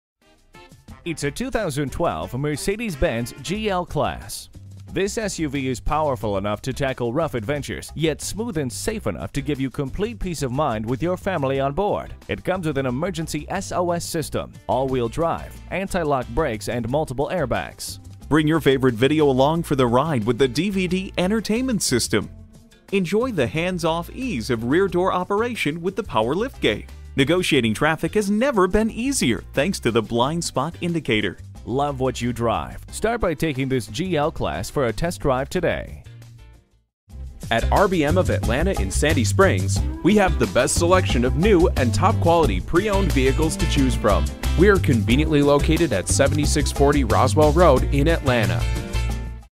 2012 Mercedes-Benz GL-Class Atlanta GA Marietta, GA U11614 - SOLD
Year: 2012
Make: Mercedes-Benz
Model: GL-Class
Trim: GL450
Engine: 8 Cyl. 4.6L
Transmission: Automatic
Color: Palladium Silver Metallic
Interior: Black
Mileage: 22185
Stock #: U11614
CERTIFIED, ONE OWNER, CLEAN CARFAX! ! P01 PREMIUM PACKAGE INCLUDING POWER DRIVER SEAT AND STEERING COLUMN WITH MEMORY, TELESCOPING & TILT STEERING COLUMN. POWER FOLDING MIRRORS, COMAND W/NAVIGATION AND VOICE CONTROL, POWER LIFTGATE, 115V AC POWER OUTLET, REAR VIEW BACKUP CAMERA, AUTODIMMING MIRROR, IPOD/MP3 MEDIA INTERFACE, HD RADIO WITH SIRIUSXM, PARKTRONIC, AIRMATIC AIR SUSPENSION, HEATED FRONT SEATS, ELECTRIC 3RD ROW SEAT, 19 TWIN 5-SPOKE ALLOY WHEELS, PARKTRONIC WITH ACTIVE PARKING ASSIST, COLLISION PREVENTION ASSIST, CLASS IV TRAILER HITCH, PRE-WIRING FOR REAR SEAT ENTERTAINMEN, BLIND SPOT ASSIST, HEATED STEERING WHEEL, BRUSHED ALUMINUM RUNNING BOARDS, AND BRUL WALNUT WOOD TRIM.

Address:
7640 Roswell Rd